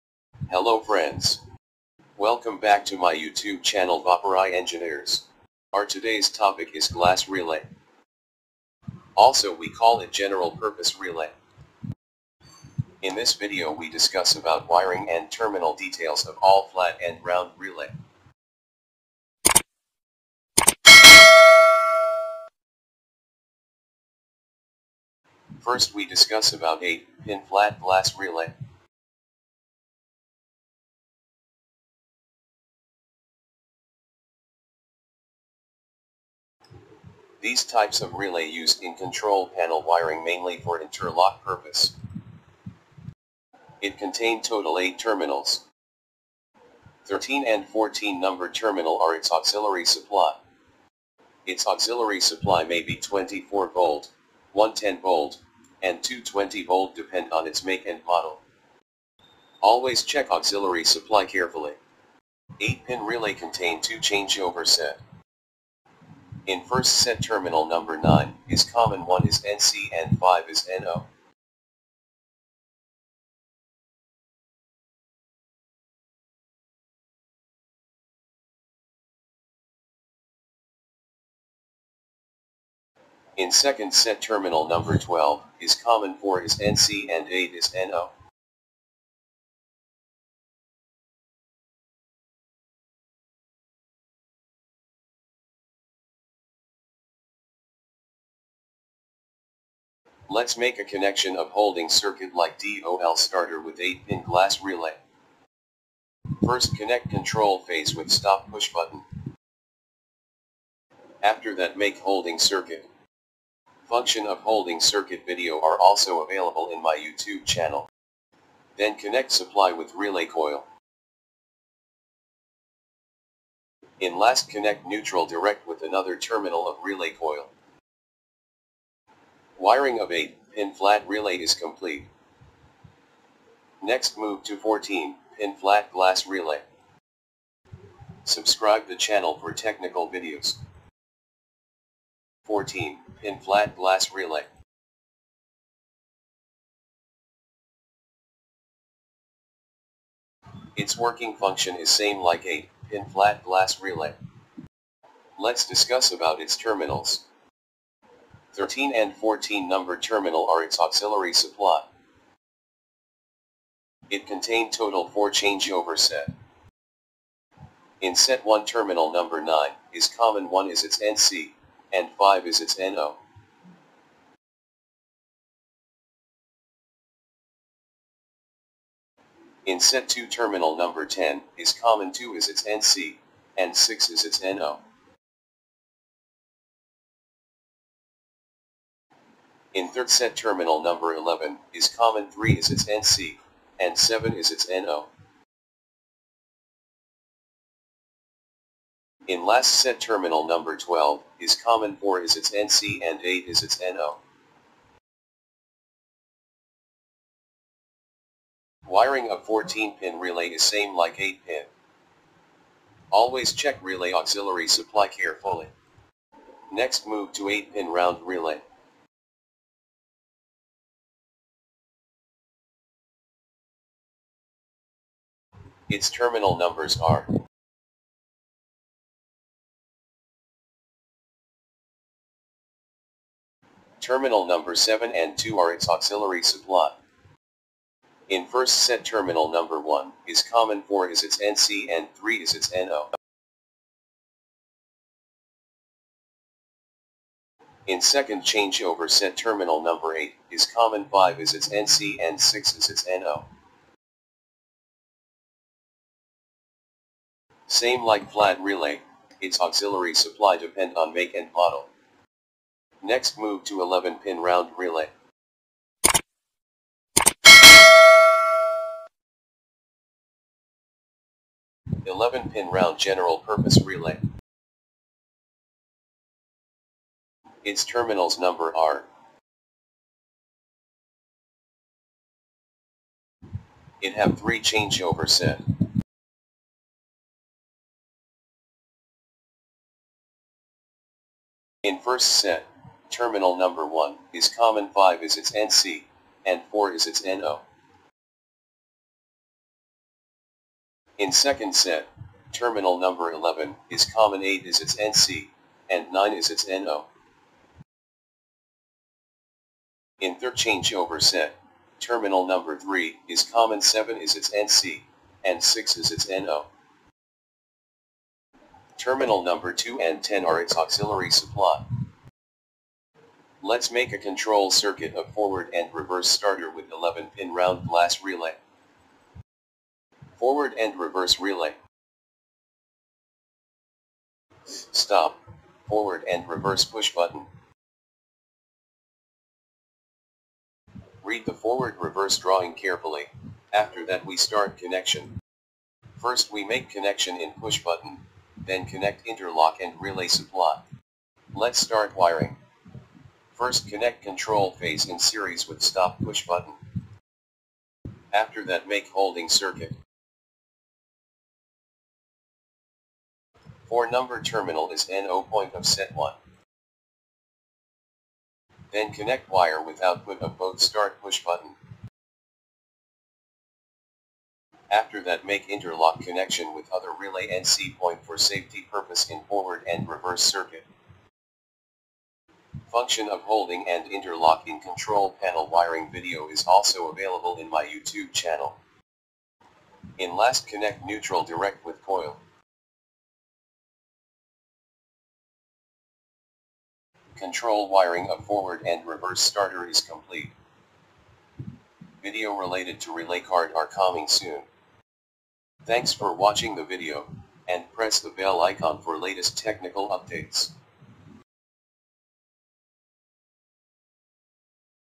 WIRING & TERMINALS DETAIL OF GLASS RELAY & MULTI PURPOSE RELAY 8PIN, 11PIN, 14PIN BOTH FLAT & ROUND
WIRING & TERMINALS DETAIL OF GLASS RELAY & MULTI PURPOSE RELAY  8 PIN FLAT WITH BASE
14 PIN FLAT WITH BASE
8 PIN ROUND WITH BASE
11 PIN ROUND WITH BASE